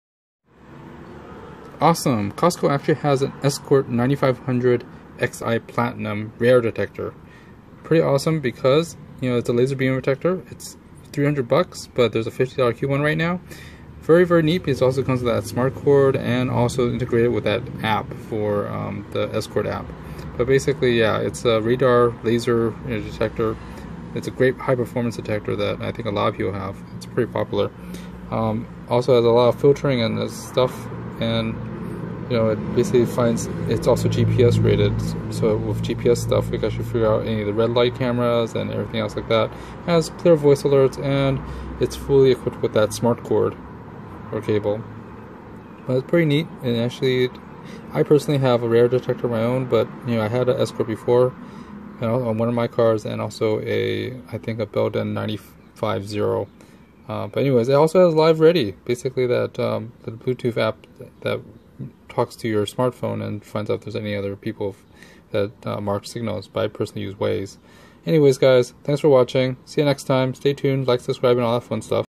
Costco! Escort 9500iX Radar Detector w/ Smart Cord $299! ($249 w/coupon!)
Costco has the Escort 9500iX Radar Detector w/ Smart Cord $299! ($249 w/coupon!)